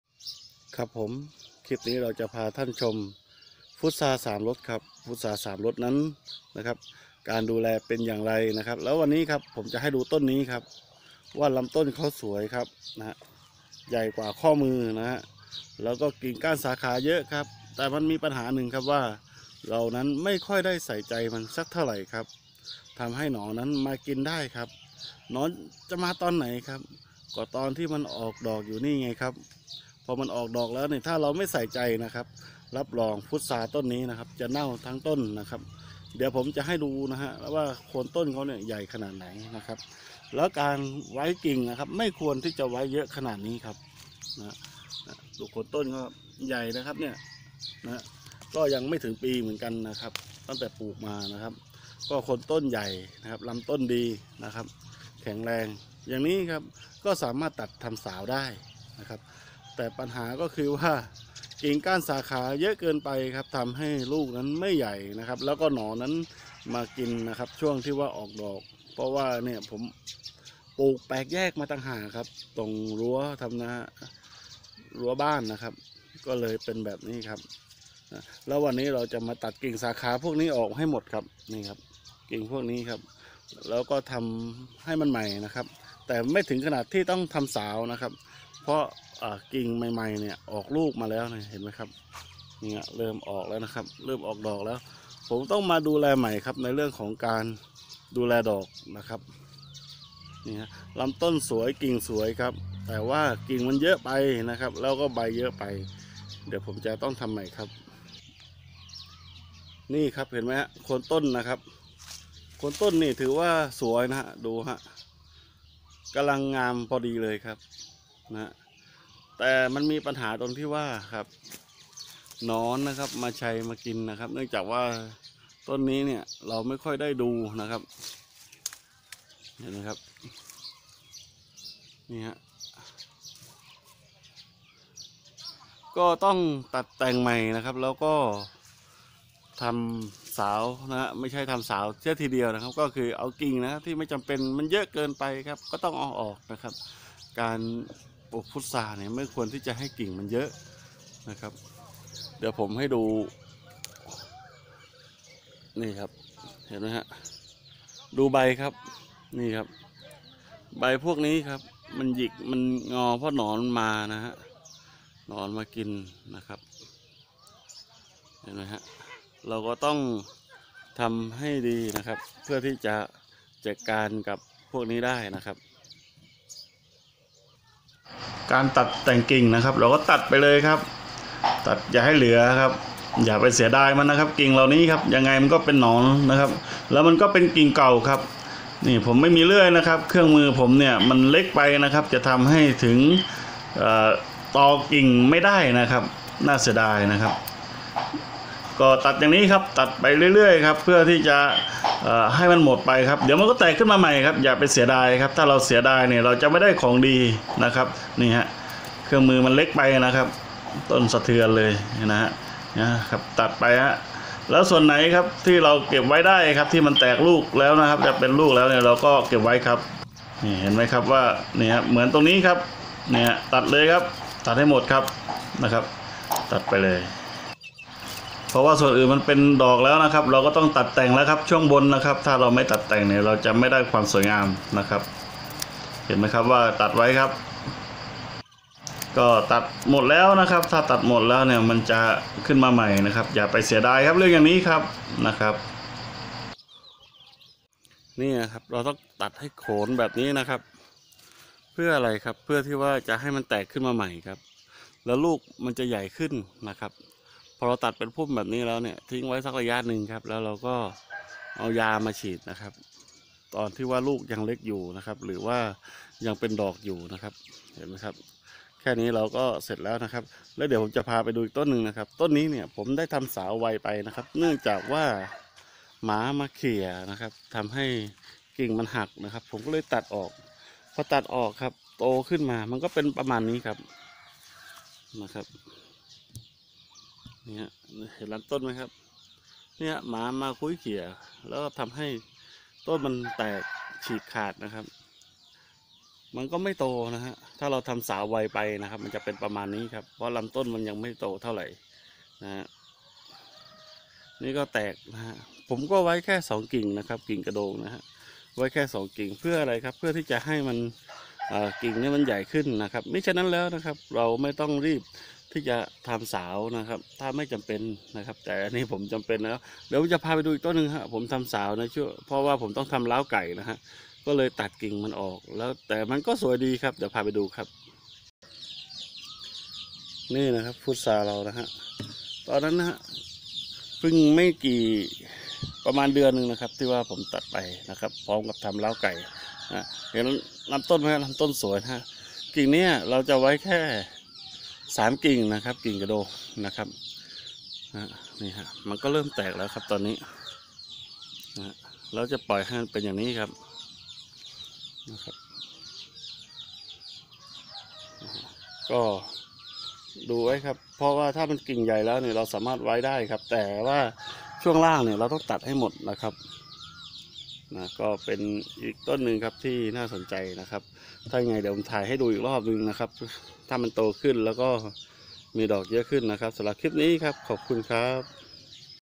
ปลูกพุทราสามรส ตัดแต่งกิ่ง พุทรา ลูกใหญ่ amazing thailand
เราต้องตัดแต่งกิ่งเมื่อที่จะให้ออกลูกอีกรอบและจะทำให้ลำต้นใหญ่